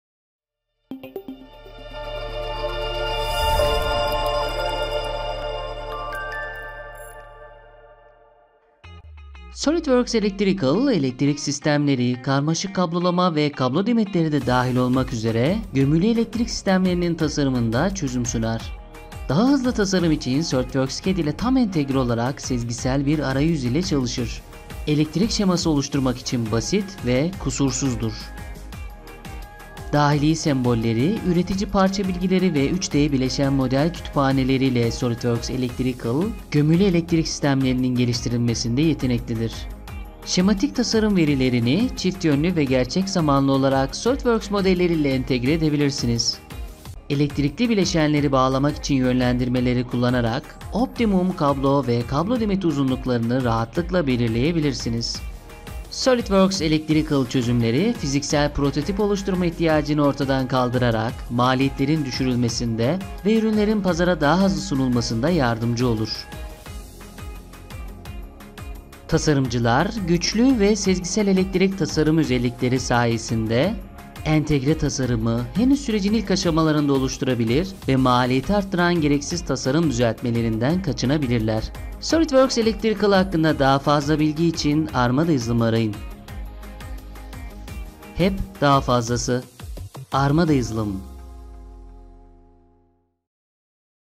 SOLIDWORKS Electrical | SOLIDWORKS Ürünleri
SOLIDWORKS Electrical; elektrik sistemleri , karmaşık kablolama ve kablo demetleri de dahil olmak üzere, gömülü elektrik sistemlerinin tasarımında çözüm sunar. Daha hızlı tasarım için SOLIDWORKS 3D CAD ile tam entegre olarak, sezgisel bir arayüz ile çalışır. Elektrik şeması oluşturmak için basit ve kusursuzdur.